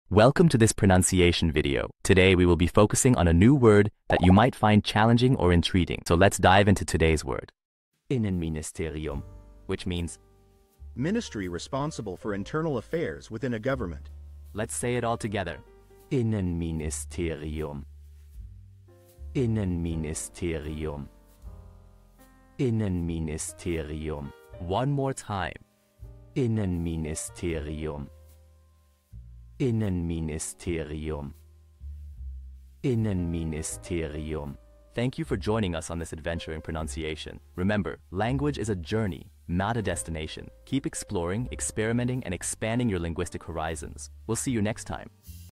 How to pronounce Innenministerium
Master the Pronunciation of 'Innenministerium which Means Ministry of the Interior' - which means : Ministry Responsible for Internal Affairs Within A Government. 📝 with @PronunciationGerman 🎓🗣️ - [Your Guide to German]

Learn to pronounce 'Innenministerium' perfectly in German with our easy and engaging tutorial by @PronunciationGerman! 📖📽️ In this comprehensive video, we provide you with native speaker examples 🗣️👂 and phonetic breakdowns 🎵 to ensure you get the pronunciation just right.

How to say Ministry of the Interior in German